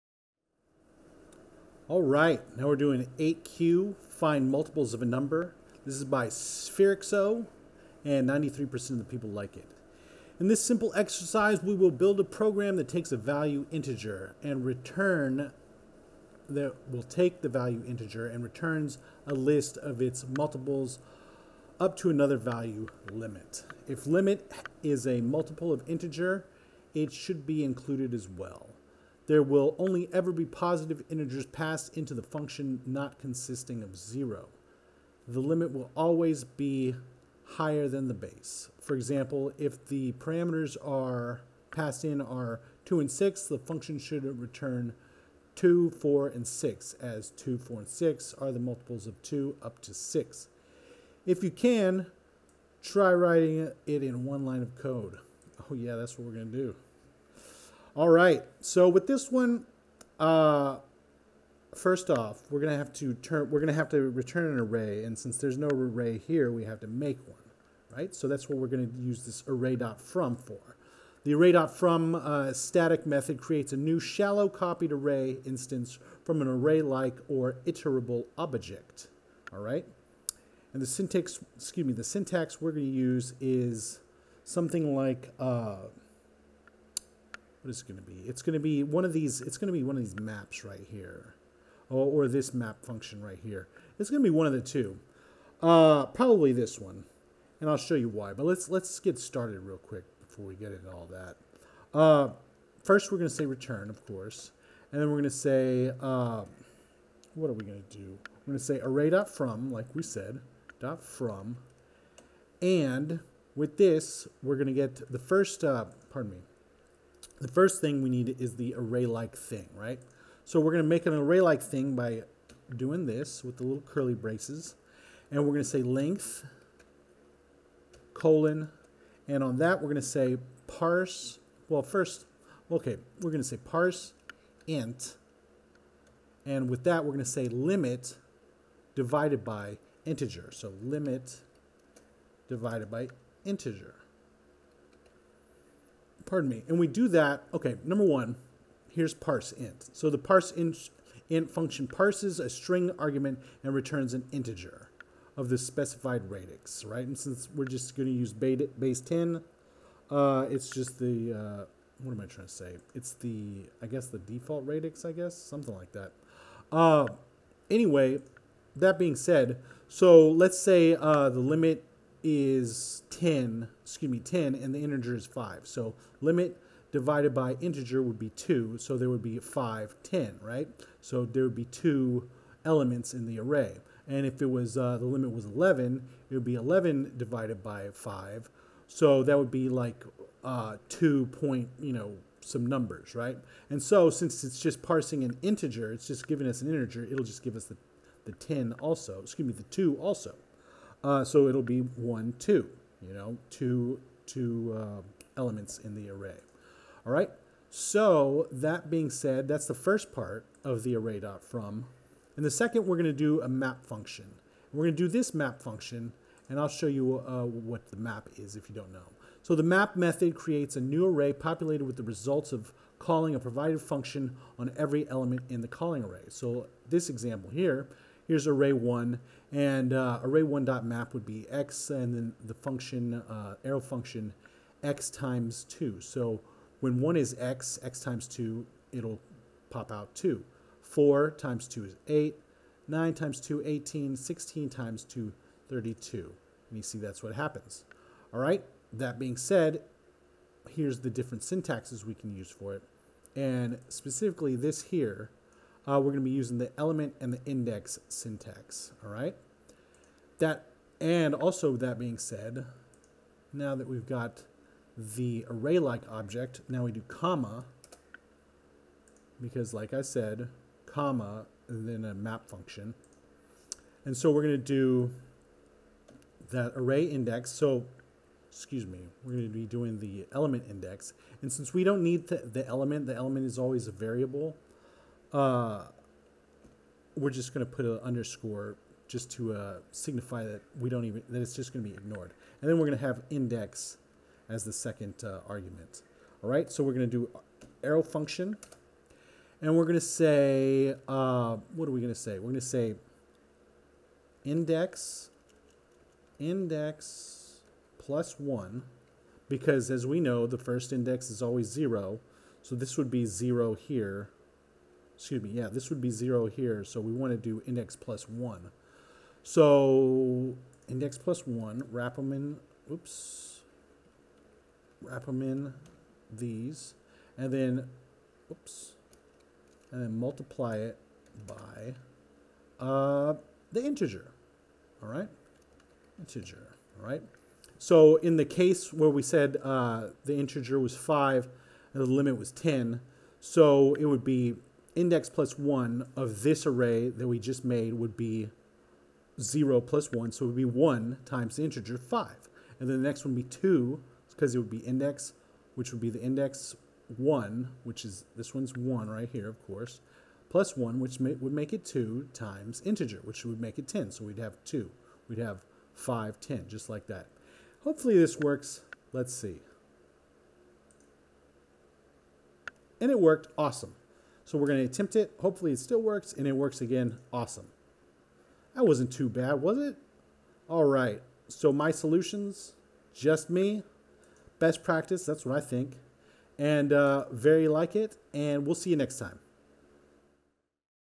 (Archived) Codewars 8 kyu Find Multiples of a Number JavaScript
this one's got Array.from(), map(), and parseInt()